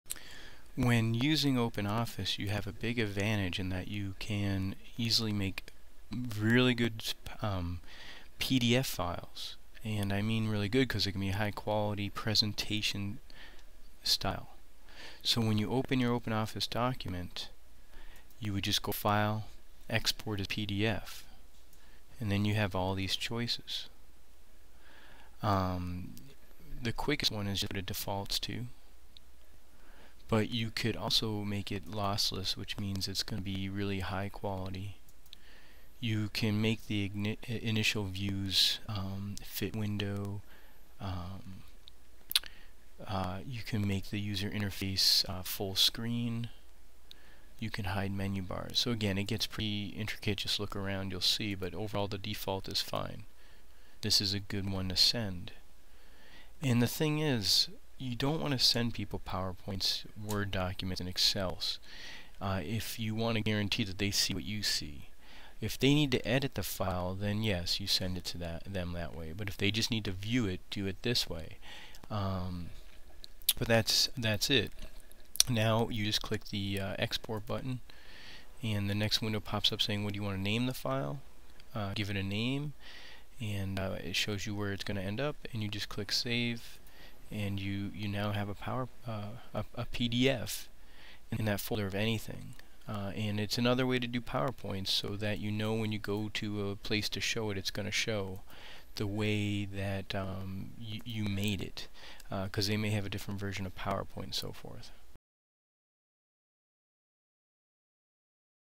PDF Export OpenOffice
How to create pdf files in OpenOffice